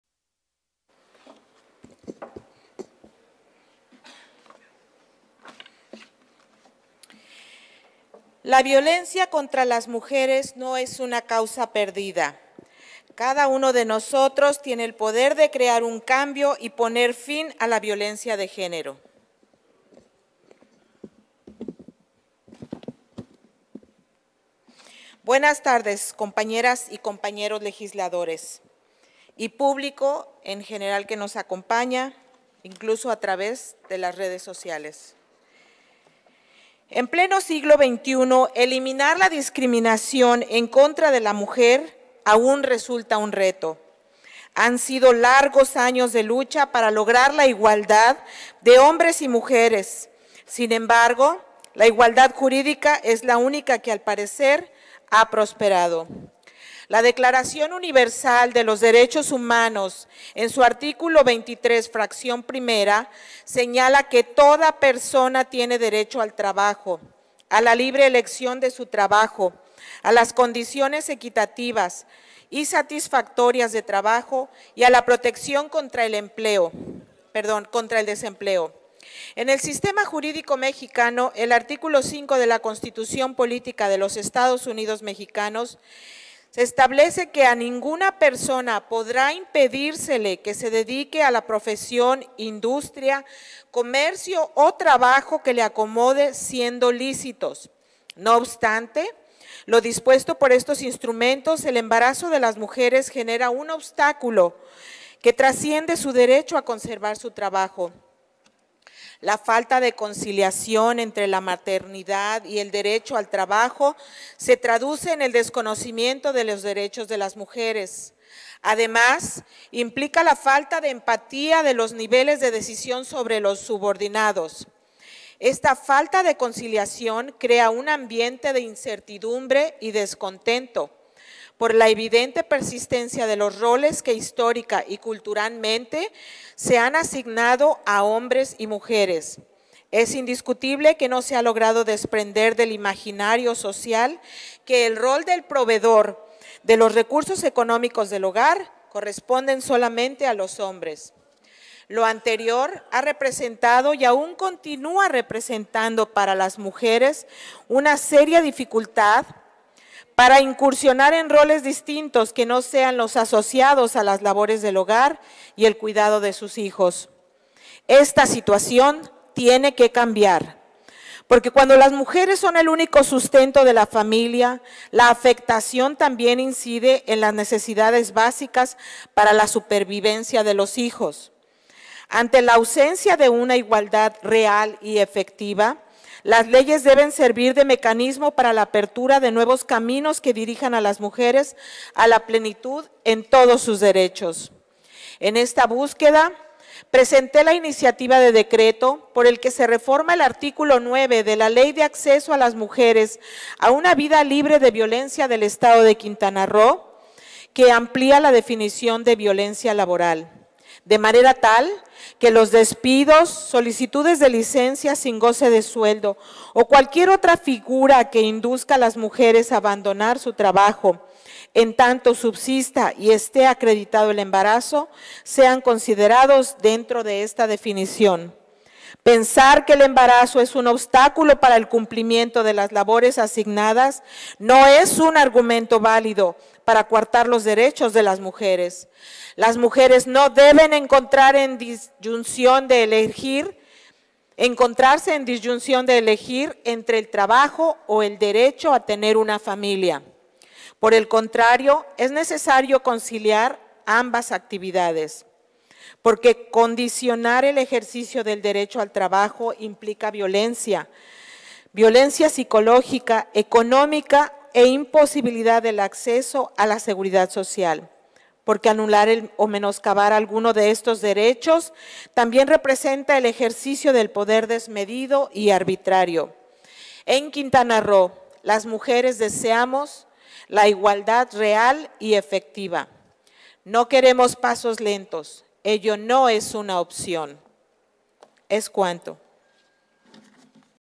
Intervención Dip. Maritza Deyanira Basurto Basurto. MC (11/Abril/2023)
Intervención de la Dip. Maritza Deyanira Basurto Basurto, de MC, en relación a la Iniciativa de decreto por el que se reforma el Artículo 9 de la Ley de Acceso de las Mujeres a una Vida Libre de Violencia del Estado de Quintana Roo; presentada por la Diputada Maritza Deyanira Basurto Basurto, integrante de la H. XVII Legislatura del Estado de Quintana Roo. Publicada en la Gaceta Parlamentaria Año I, número 45 Ordinaria, de fecha 04 de abril de 2023.